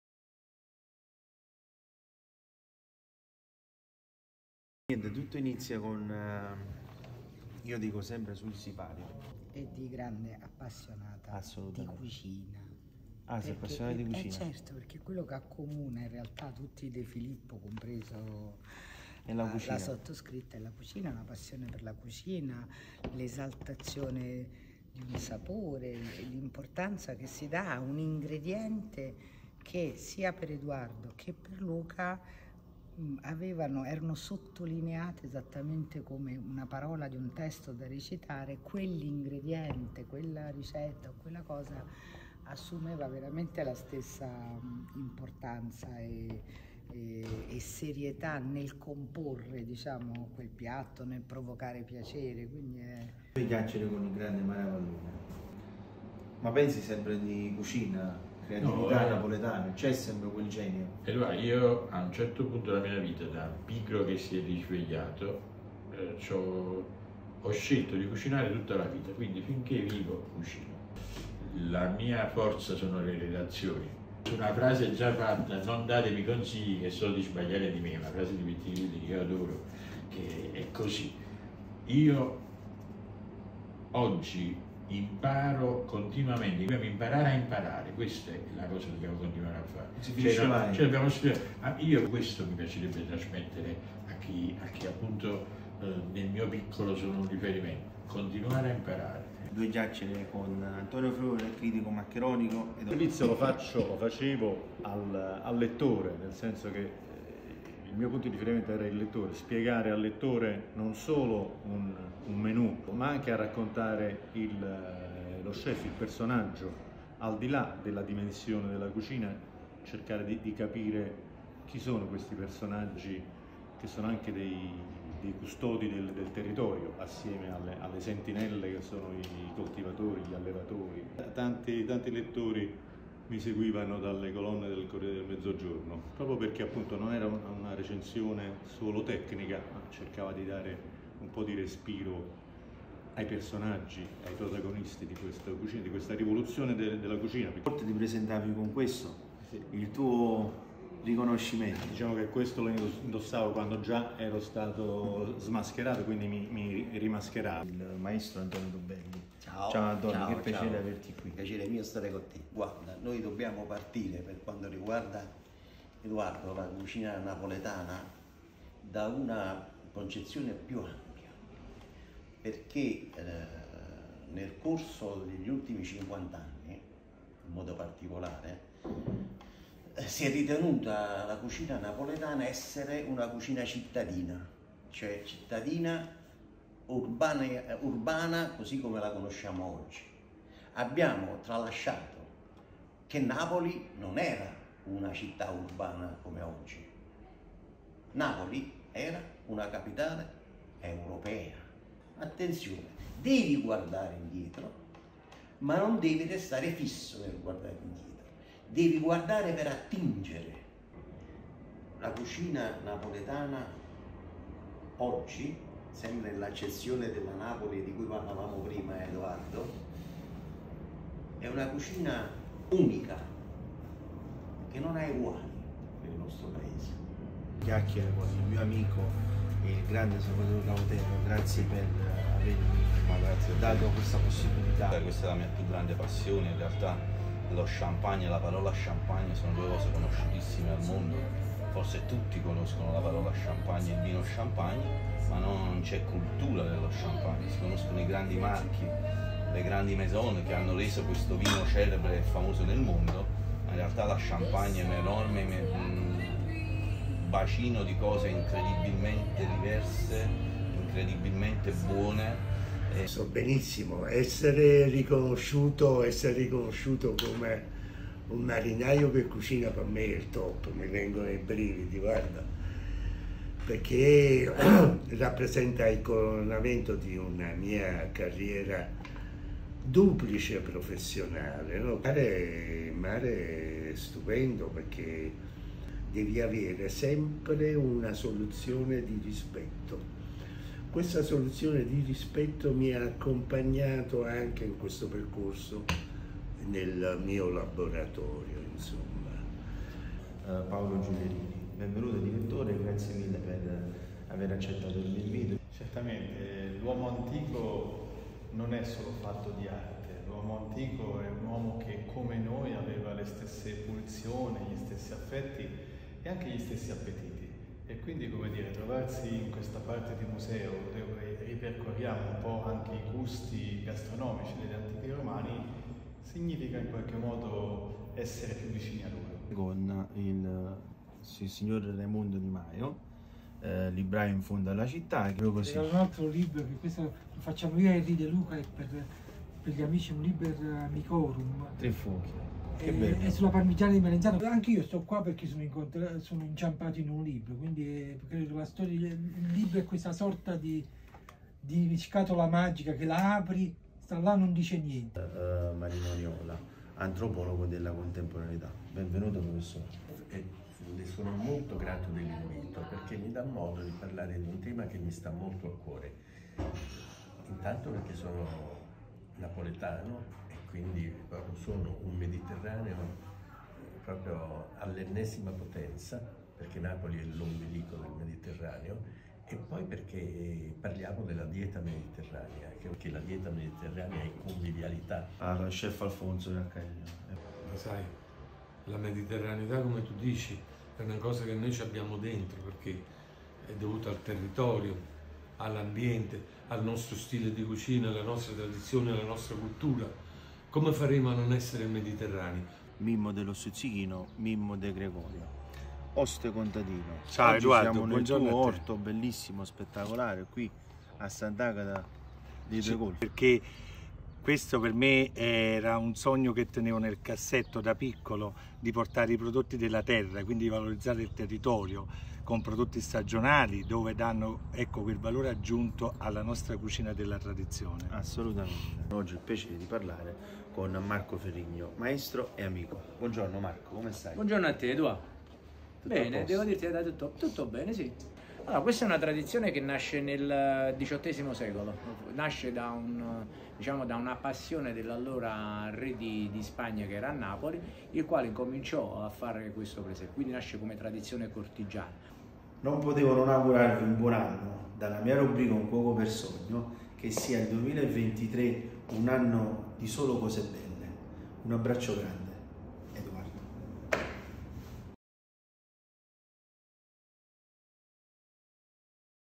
Un cuoco per sogno, due chiacchiere con...
Due chiacchiere con il nuovo anno mi fanno ripensare a tutte le persone che ho conosciuto e incontrato nel corso del 2022. Ciascuna di loro ha lasciato un segno, un ricordo e un pezzetto di sé,  un riepilogo di emozioni,  buoni auspici, e buoni propositi per un 2023 prospero e ricco di amore e speranza. Auguri!